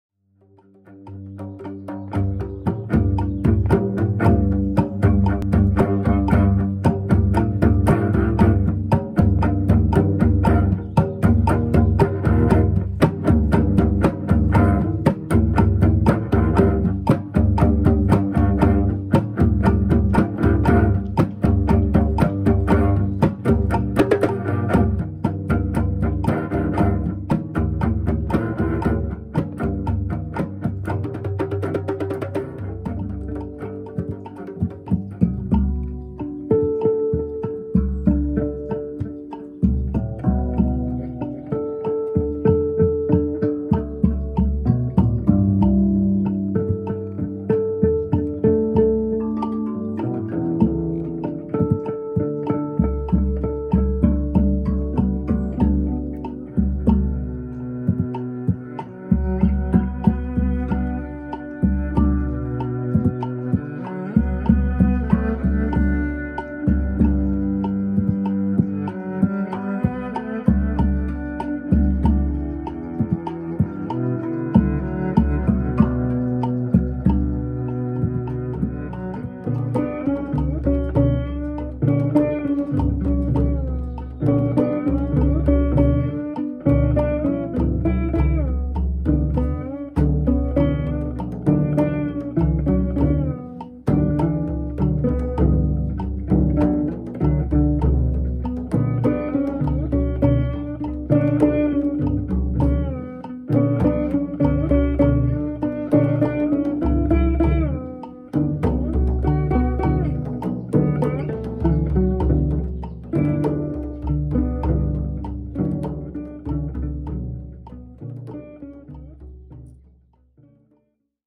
Kim Atkinson & RutiCelli
Live Duo performing locally in Sonoma County (Improvisational material)

Let us sound the spirit of your event :)